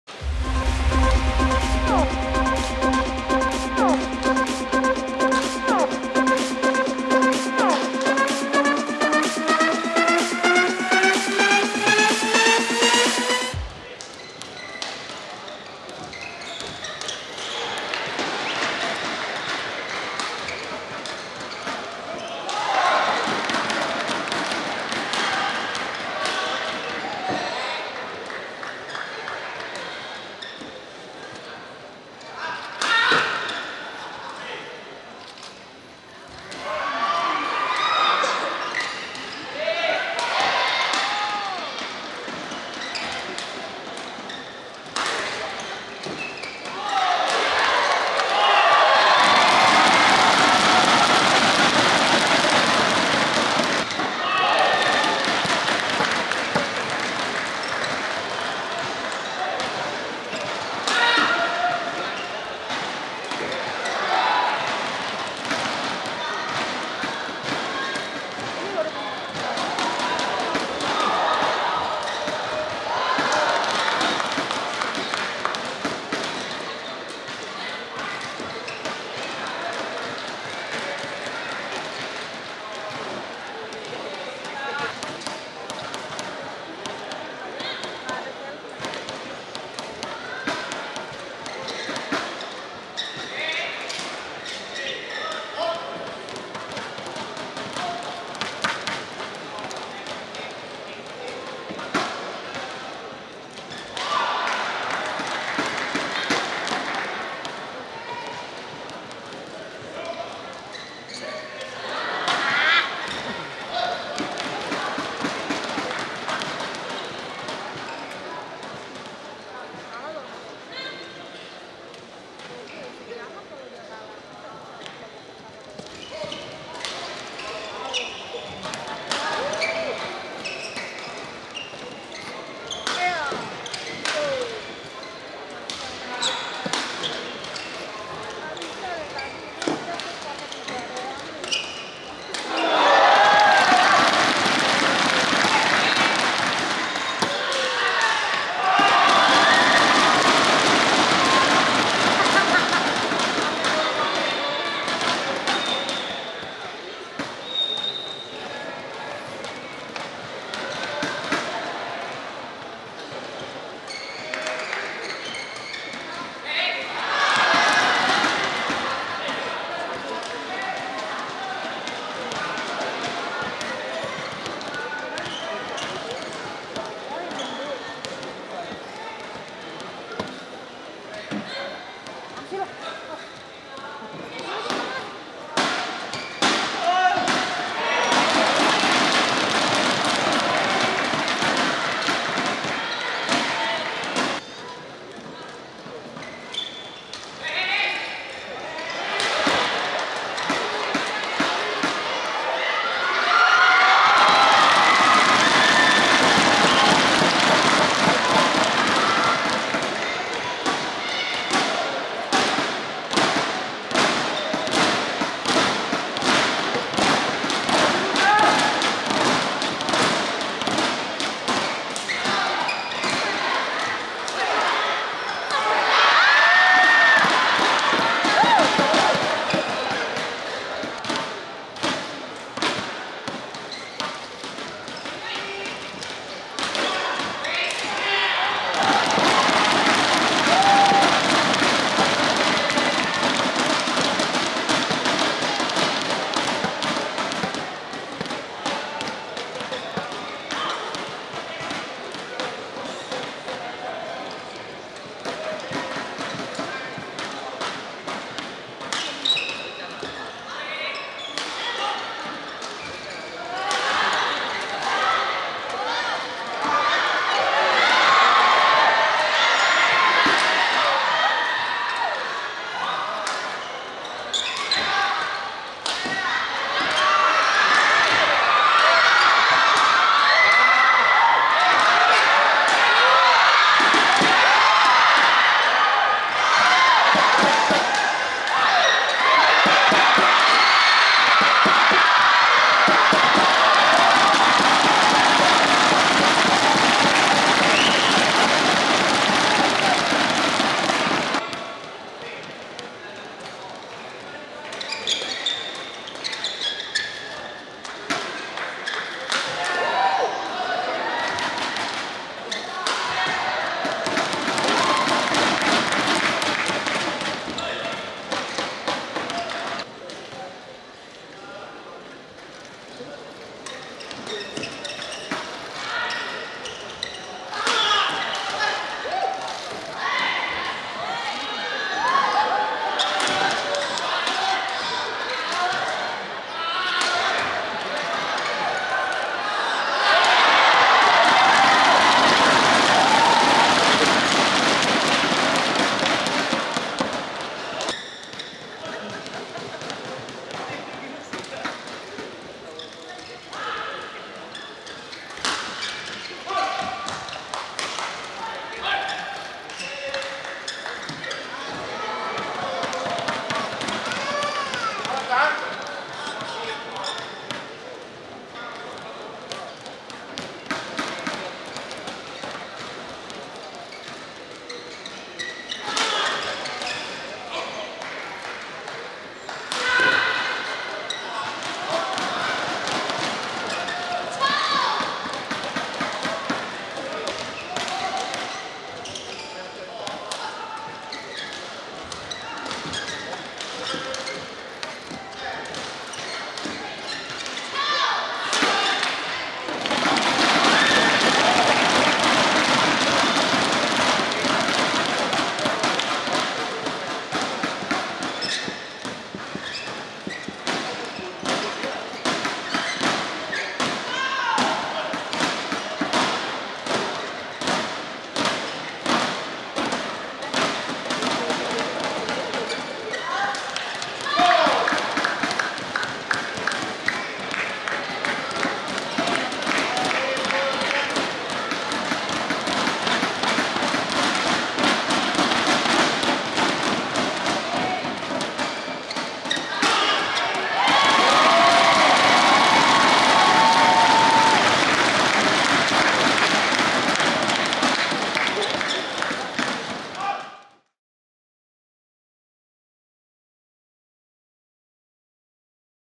Tontowi Ahmad/Liliyana Natsir (Indonesia) VS Riky Widianto/Masita Mahmudin (Indonesia)
Round 1 Mixed Doubles Daihatsu Indonesia Masters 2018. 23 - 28 Januari 2018 at Istora Senayan - Jakarta